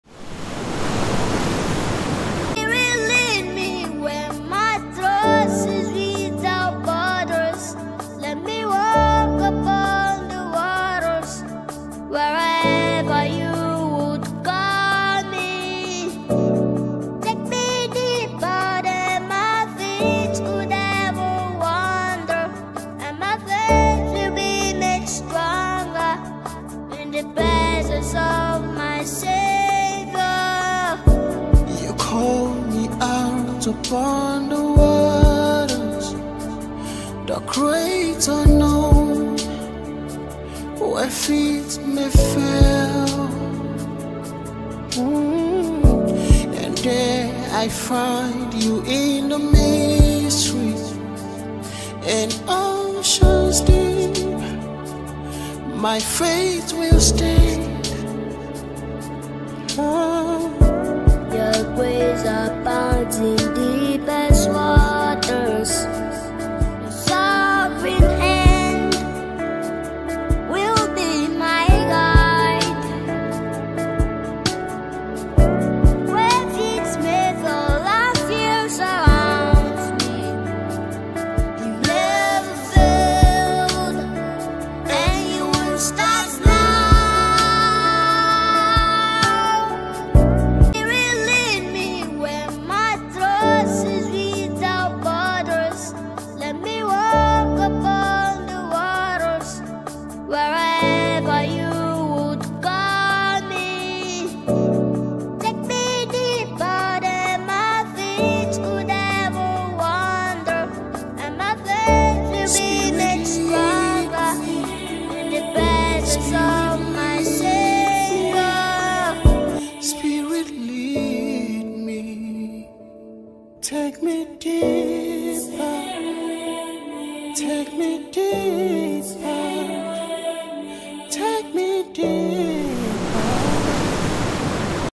World Ocean Day Drawings, Posters and Crafts
World Ocean Day is celebrated every year on 8th June.
The drawings and posters are made by tiny tots of class 1 and 2.